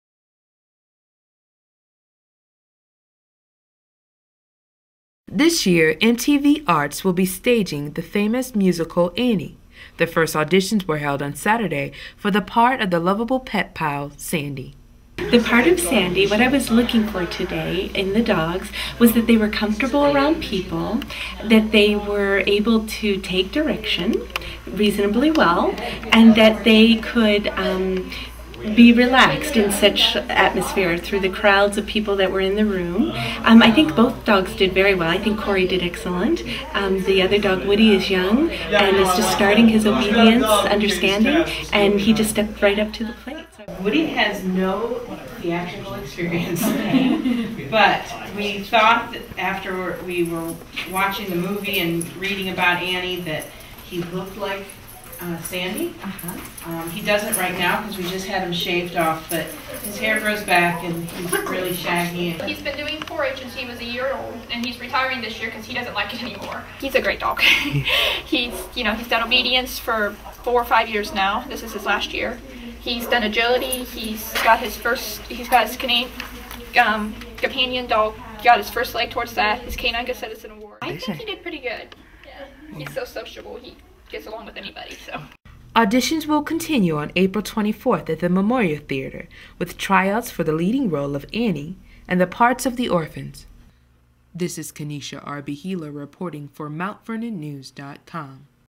Dogs audition for role in 'Annie'
MOUNT VERNON — MTV Arts is setting the stage once again with this year's production of Annie. On Saturday, auditions were held at the Harcourt Veterinary Clinic for the part of the adorable pet pal, Sandy.